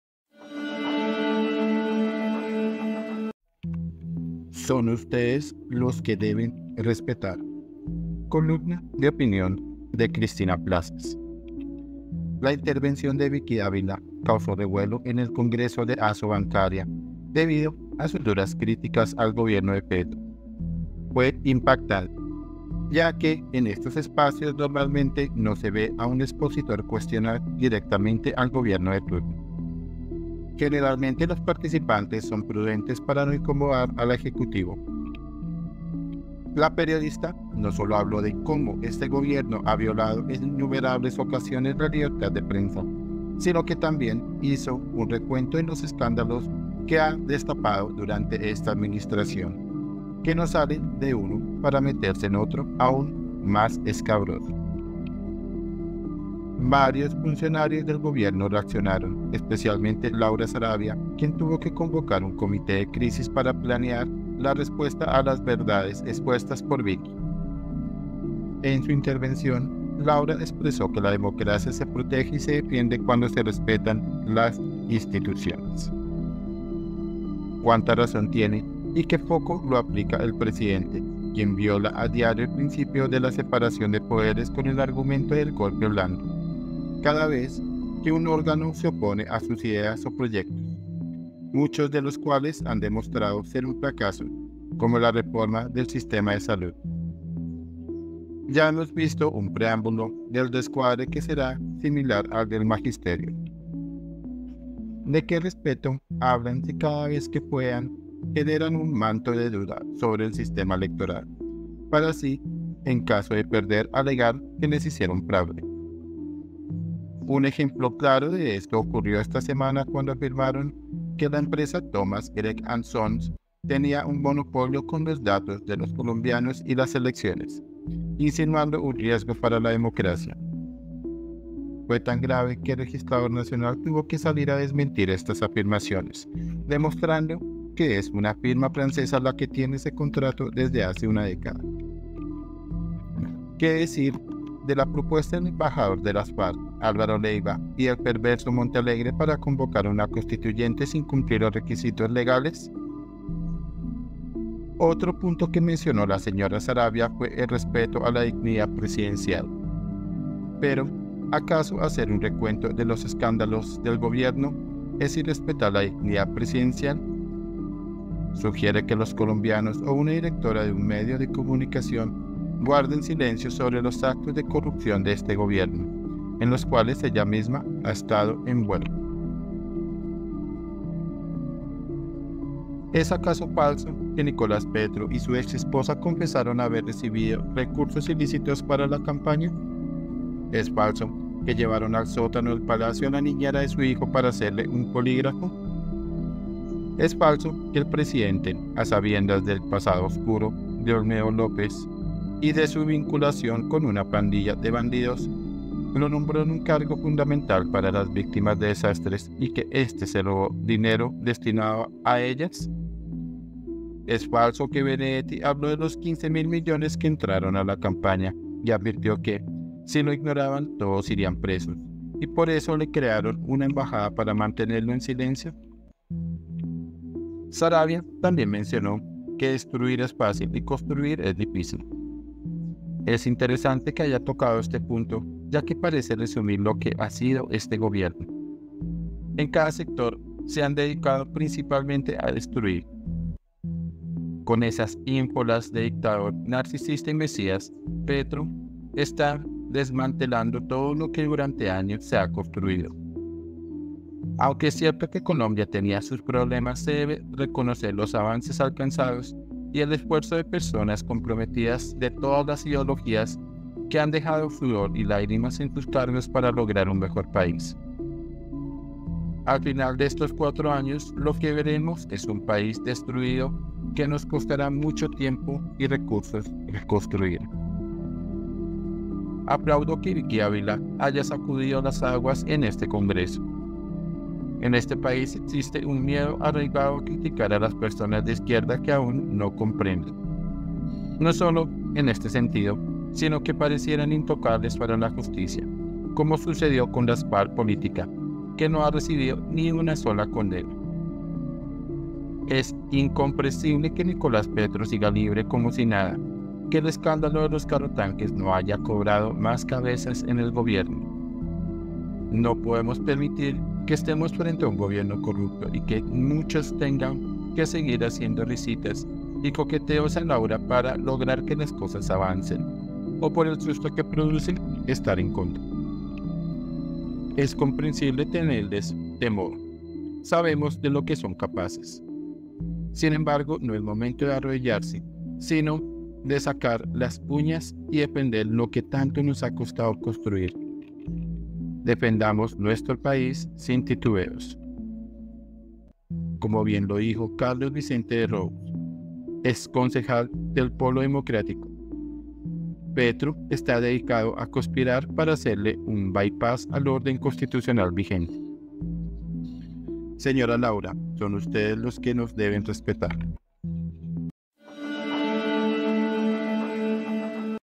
SON USTEDES LOS QUE DEBEN RESPETAR 🔴 Columna de opinión de Cristina Plazas
La intervención de Vicky Dávila causó revuelo en el congreso de Asobancaria debido a sus duras críticas al gobierno de Petro. Fue impactante, ya que en estos espacios normalmente no se ve a un expositor cuestionar directamente al gobierno de turno. Generalmente, los participantes son prudentes para no incomodar al ejecutivo.
columna CRISTINA PLAZAS para VANGUARDIA